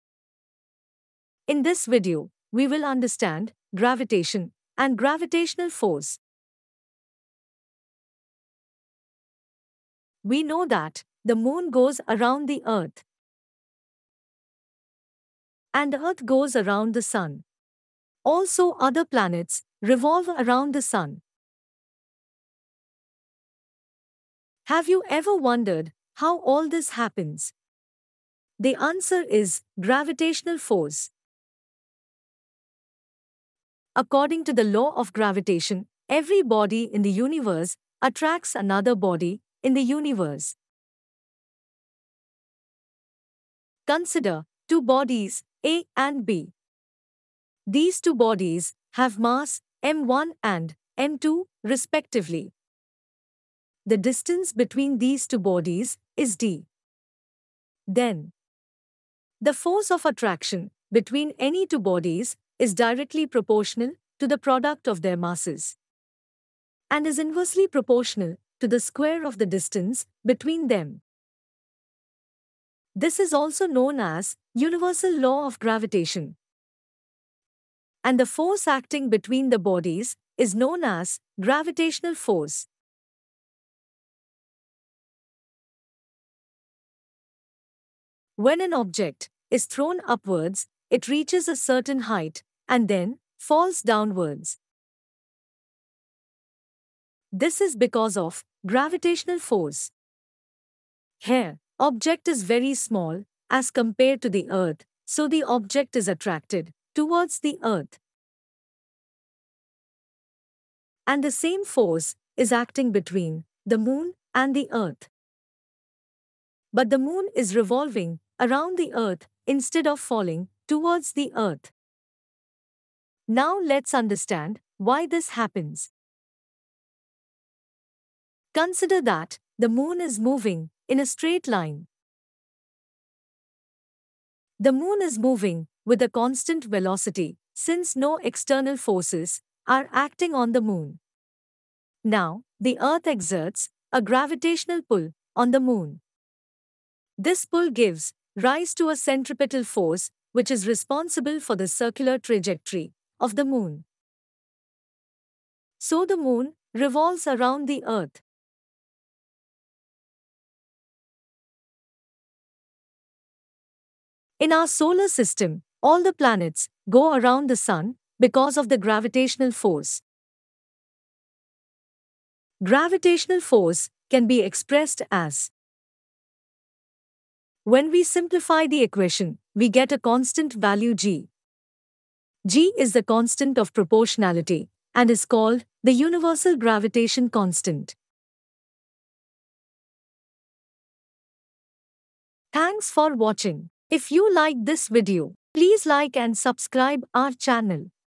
Gravitation || 3D animated explanation || class 9th
Gravitation, also known as gravity, is a fundamental force of nature that governs the motion of objects with mass. It is described by Sir Isaac Newton's law of universal gravitation and is a key concept in physics and astronomy.